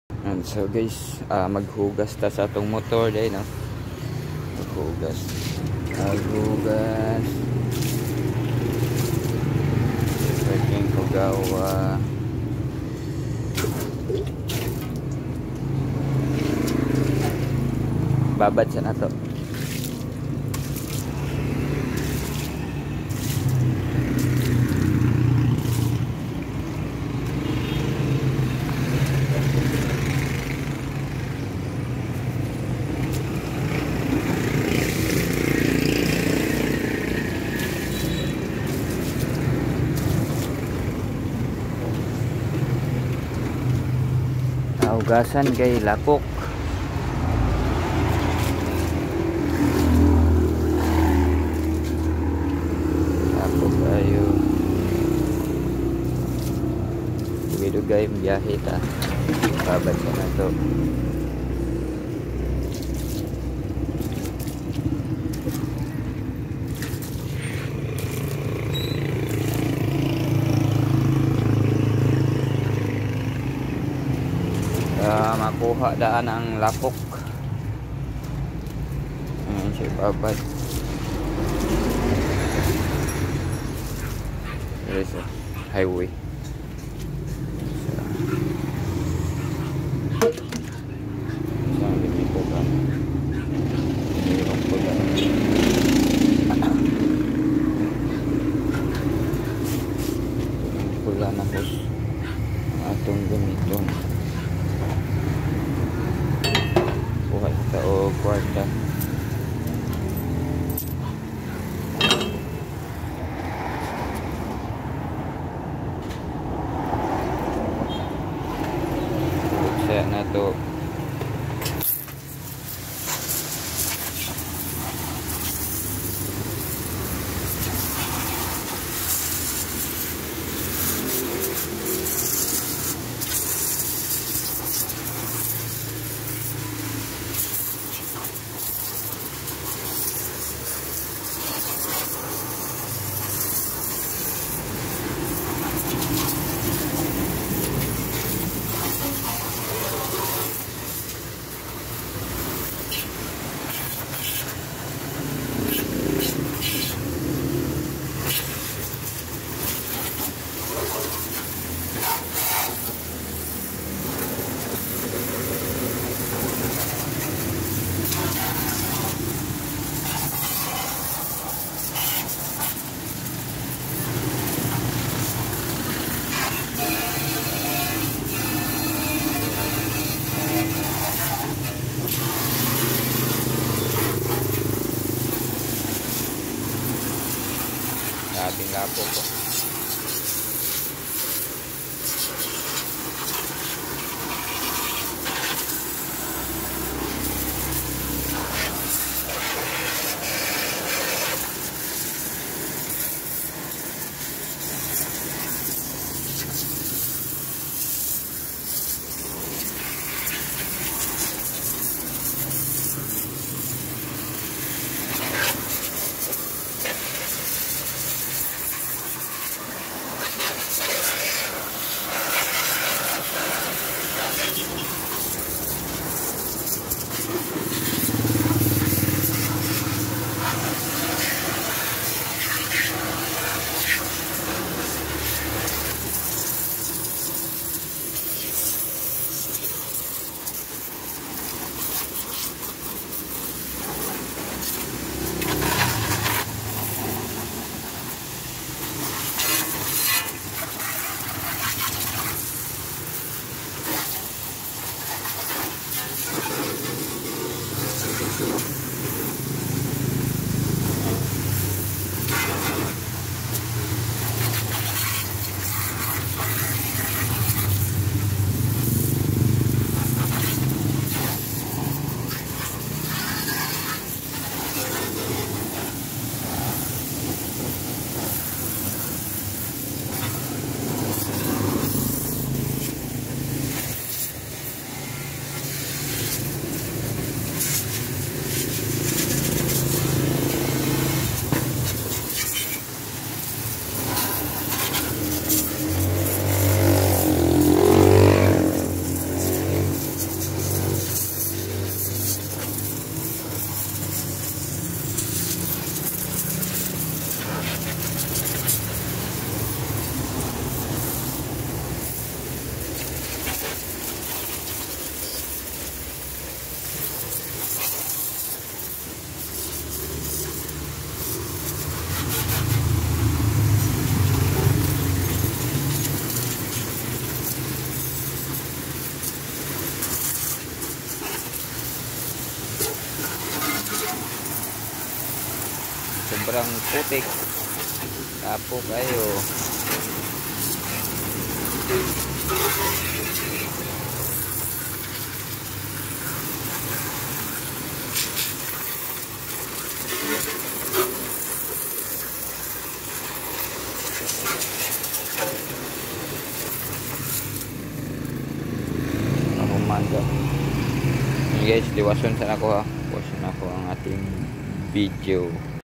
hugas muna sating motor
Linis muna tayo sating motor ang ating pinag kukunan ng pangkain sa araw araw